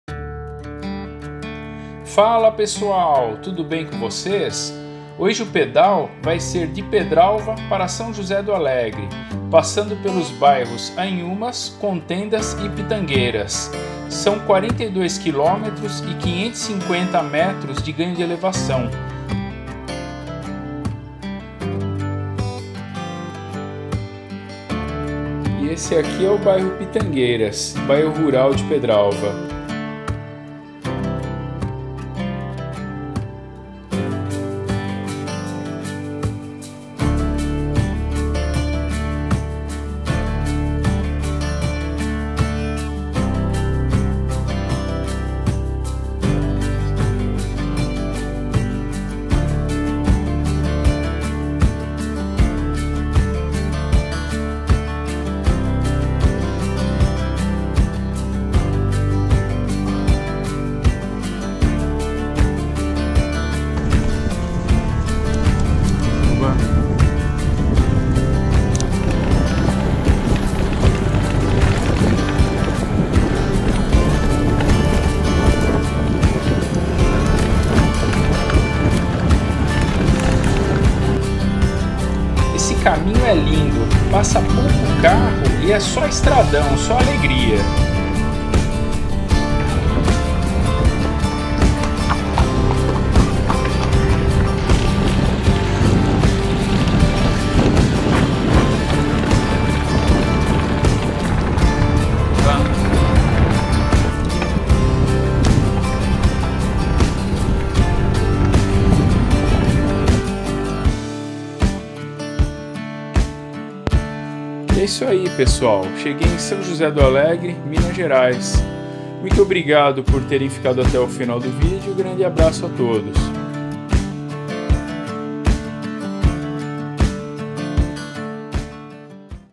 Pedal de Pedralva até São José do Alegre, MG
Pedal com destino à Cidade de São José do Alegre, passando por
Anhumas, Contendas e Pitangueiras (bairros rurais de Pedralva, MG).
Trajeto com 42km e ganho de elevação de 550m

Video gravado em 12 de abril de 2020.